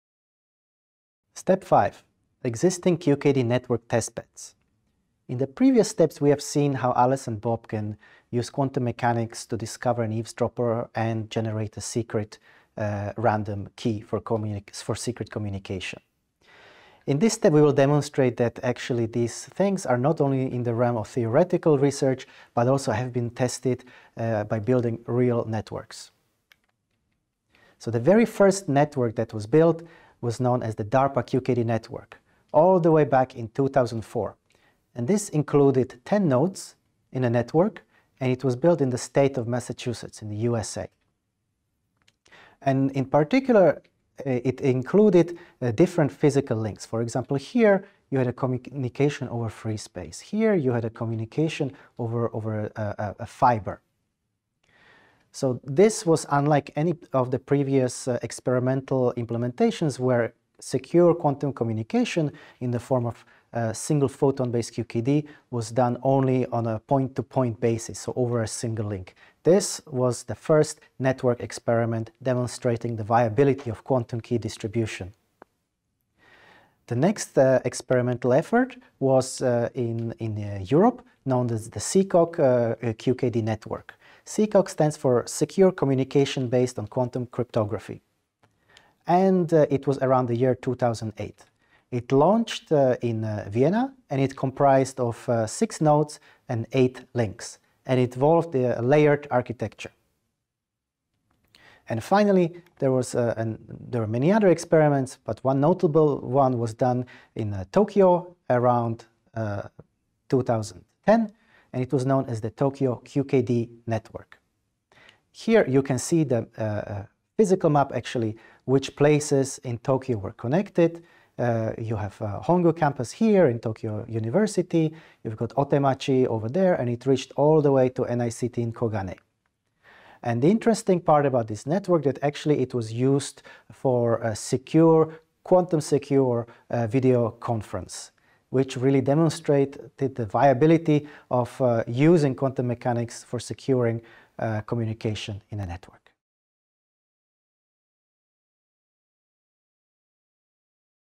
9-5 Existing QKD network testbeds
Lesson 9 BB84: Single-photon QKD
Step 5: Existing QKD network testbed

We conclude this lesson by taking a look at some of the testbed single-photon QKD networks that were built.

This is part of our module: "Overview of Quantum Communications".